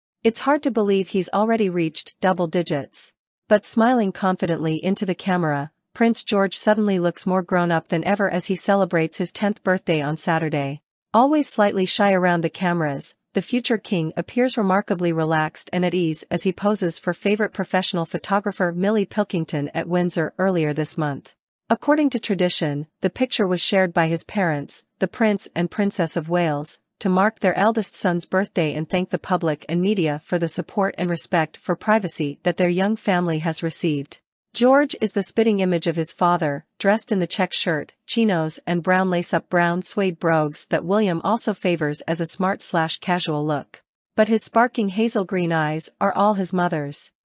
Beaming Prince George looks smart in new portraits released to mark his 10th birthday today
It’s hard to believe he’s already reached double digits.
But smiling confidently into the camera, Prince George suddenly looks more grown up than ever as he celebrates his 10th birthday on Saturday.
Always slightly shy around the cameras, the future king appears remarkably relaxed and at ease as he poses for favourite professional photographer Millie Pilkington at Windsor earlier this month.
According to tradition, the picture was shared by his parents, the Prince and Princess of Wales, to mark their eldest son’s birthday and thank the public and media for the support and respect for privacy that their young family has received.
George is the spitting image of his father, dressed in the check shirt, chinos and brown lace-up brown suede brogues that William also favours as a smart/casual look.
But his sparking hazel-green eyes are all his mother’s.